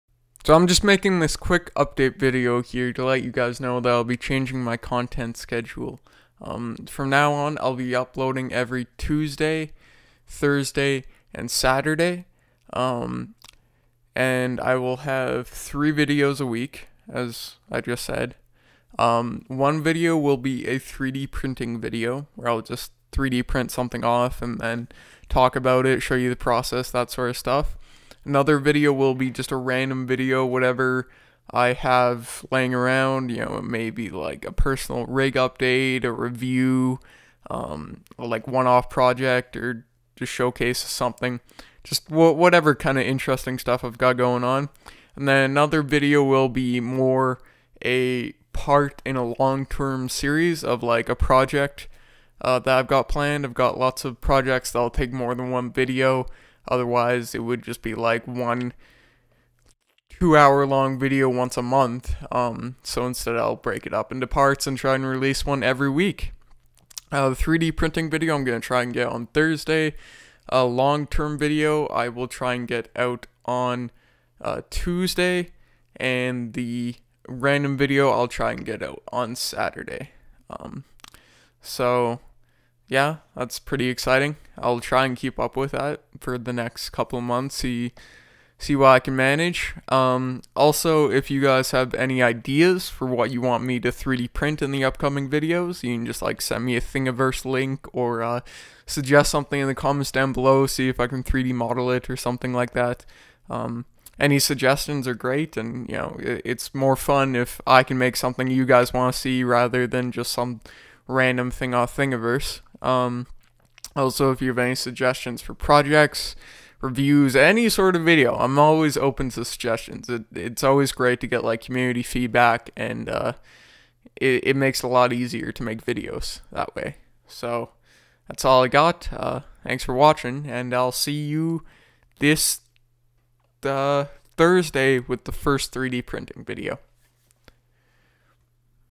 New Video Schedule and I Want Your Suggestions!
If you have anything you would like to see printed or any other video ideas leave them in the comments.